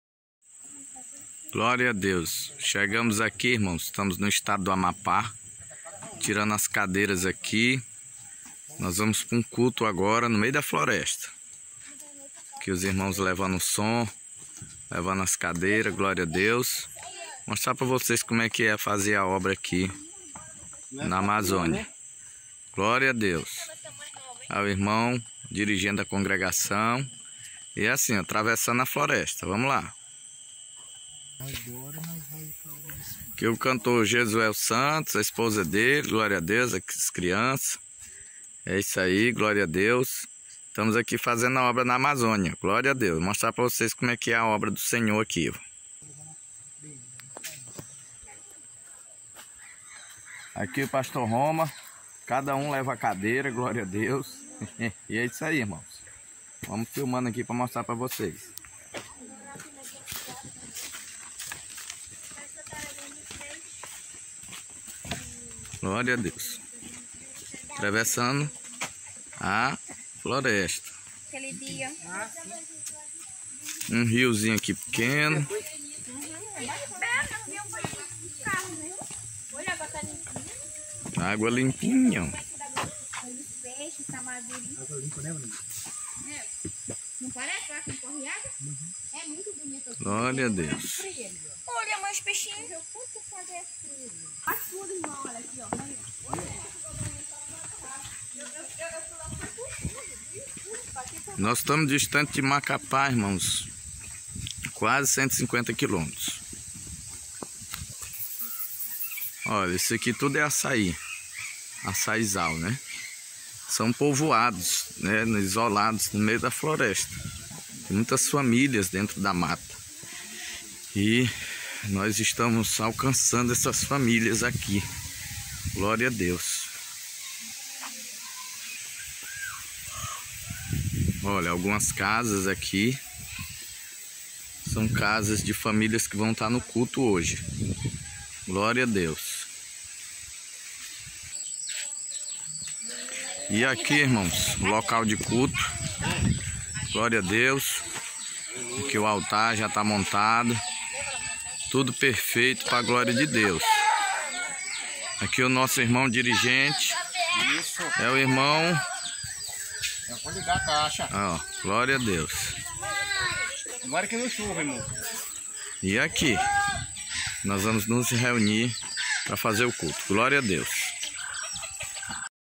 missões no Amapá com Pr Roma Martins 2021 regiao de Tracajatuba
atravessando a floresta